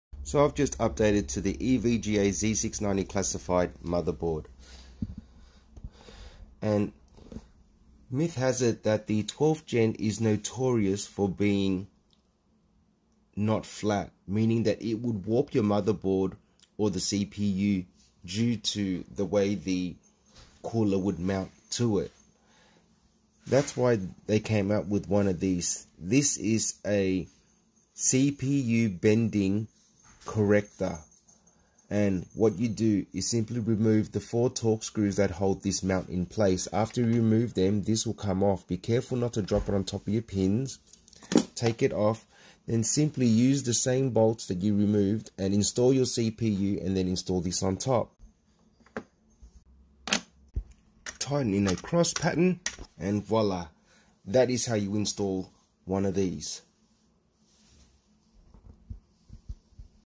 how to install 12th gen lga 1700 cpu bending corrector frame plate. prevents warping and bending
in this video i install one of these gadgets that apparently helps to keep your cpu or mobo from warping or bending  aka cpu contact frame
as stated in video i have heard that the 12th gen cpu did completely come out flat
meaning that the surface contact area of the cpu to the cooler wasnt 100%
thereby causing alot of overheating and also warping and bending of the mobo

after hearing this i wanted to try it out
so i installed it , in a nice red aluminum and we have to see in a few days how it goes, i will come back with another video and share if the mounting plate did help or not